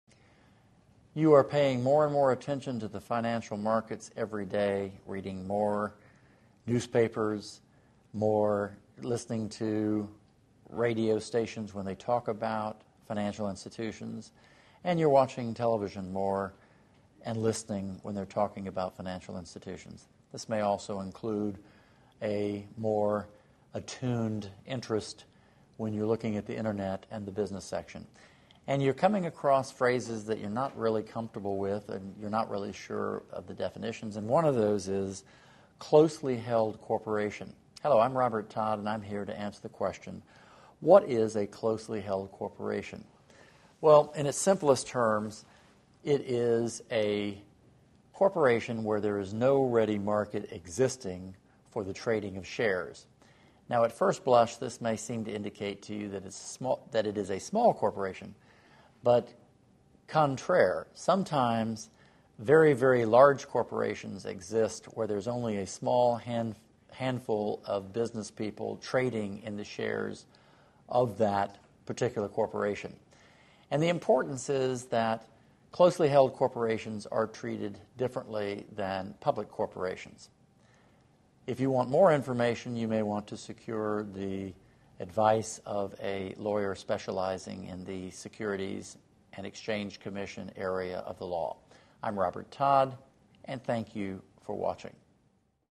What Is a Closely Held Corporation?
What Is a Closely Held Corporation?. Part of the series: Business Laws. A closely held corporation is a business or corporation where there is no ready market existing for the trading of shares. Find out more about how closely held corporations are treated differently than public corporations with tips from a lawyer in this free video on business law.